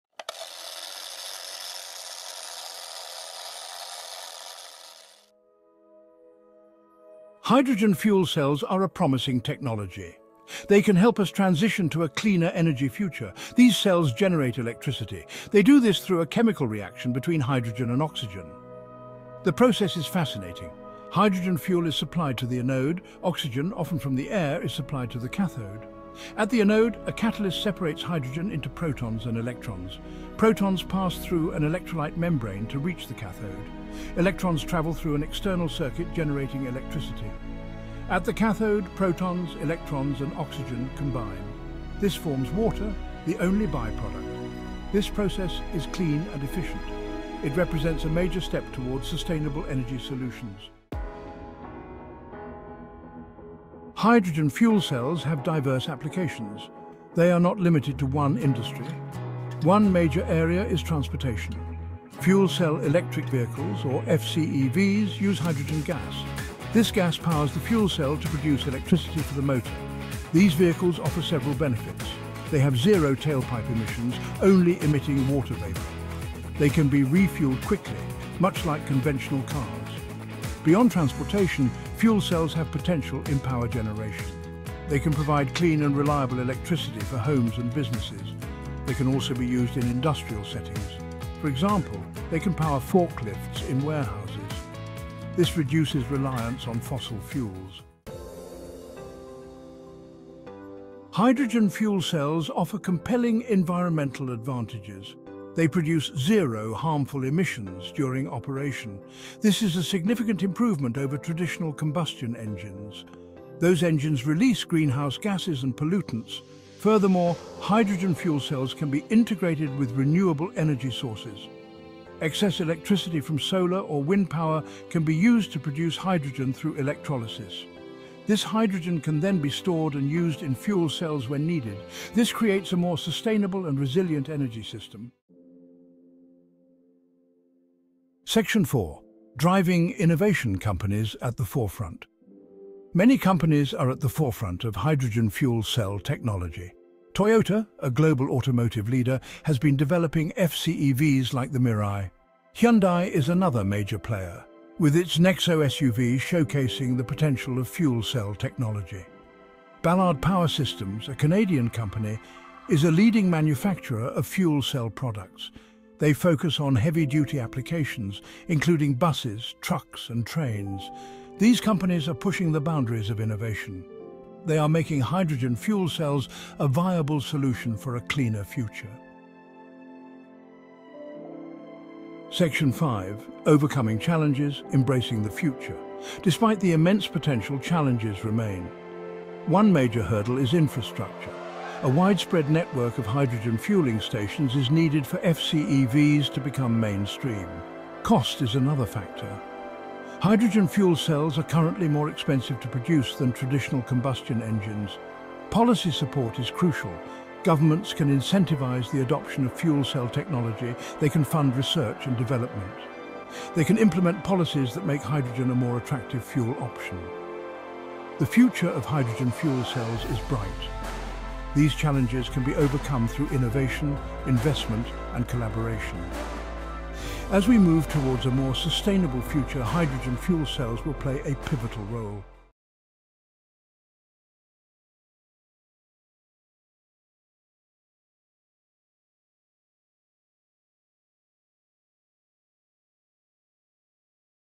Hydrogen Fuel Cells: Powering a Clean Energy Future | EcoInnovaTech.
Today, we’re diving into the innovative world of **hydrogen fuel cells**—a technology that promises to power a clean energy future. Imagine a world where our energy needs are met with zero emissions. Join us as we explore how hydrogen fuel cells are revolutionizing various industries and contributing to a sustainable future.

- **What are Hydrogen Fuel Cells?**
  Learn the science behind hydrogen fuel cells and how they generate clean energy.
- **Applications of Hydrogen Fuel Cells**
  Discover how hydrogen fuel cells are being used in transportation, power generation, and industrial processes.
- **Environmental Benefits**
  Understand the environmental advantages of hydrogen fuel cells, including zero emissions and integration with renewable energy sources.
- **Current Developments and Innovations**
  Explore the latest advancements from leading companies like Toyota, Hyundai, and Ballard Power Systems.
- **Challenges and Future Prospects**
  Discuss the challenges in scaling hydrogen fuel cells, infrastructure development, cost, and policy support.

Stay informed with **EcoInnovaTech** for the latest in tech innovations and sustainable solutions.